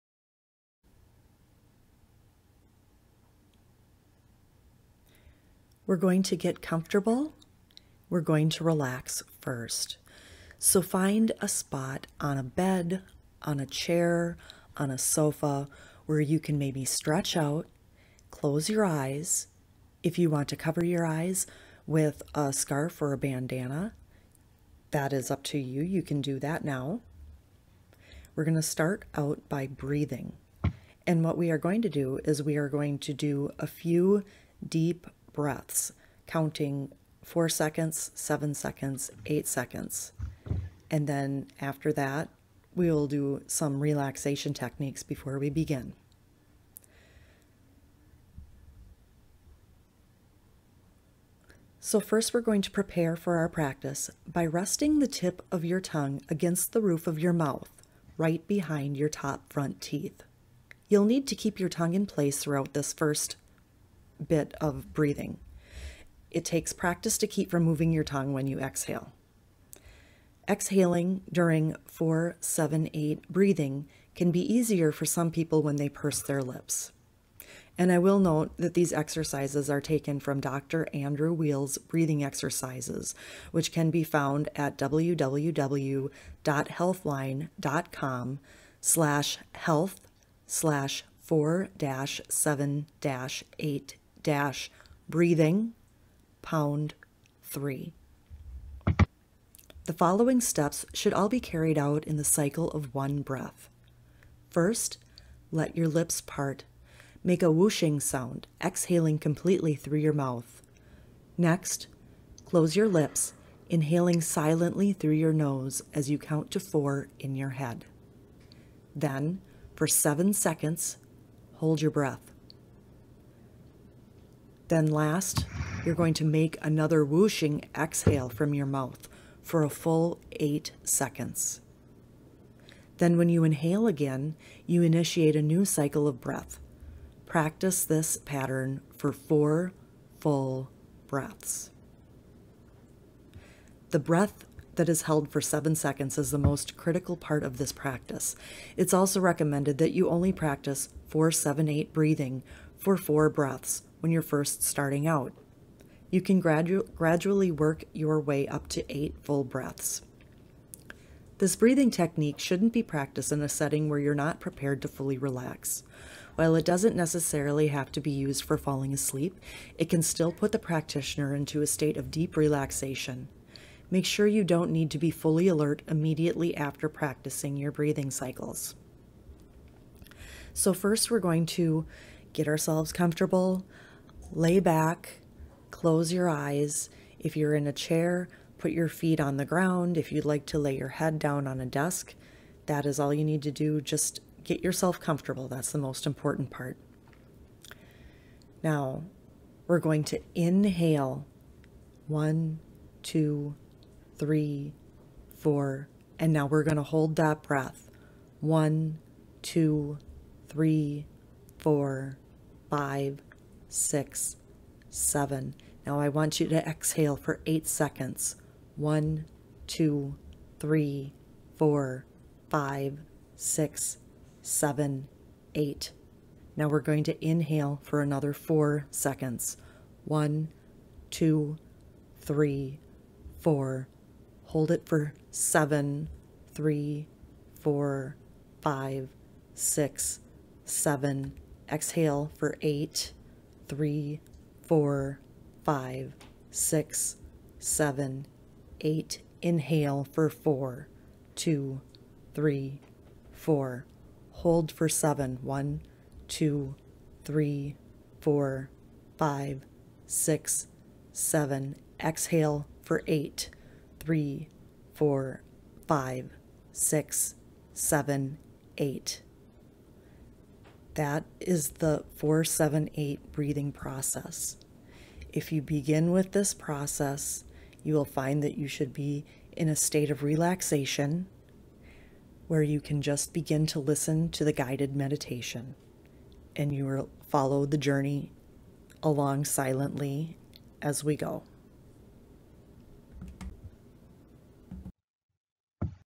4-7-8 Breathing
Meditation has been a part of my daily practice for nearly 40 years now.  I began giving guided meditations in 2018, and have recently decided to record some of these so that I can share them with others.

This technique may be useful to anyone wishing to focus on breathing for relaxation, meditation or anxiety.  It's a technique that can be useful for humans of any age, who are able to count.

As breathing is crucial to relaxation, I'm posting this video first as it's the one thing that a lot of people don't really think about if they've never meditated before.  The relaxation techniques "we will do later" which I mention during this video are actually a part of the second video I will post after this one, which is a Journey to Meet Your Inner Child.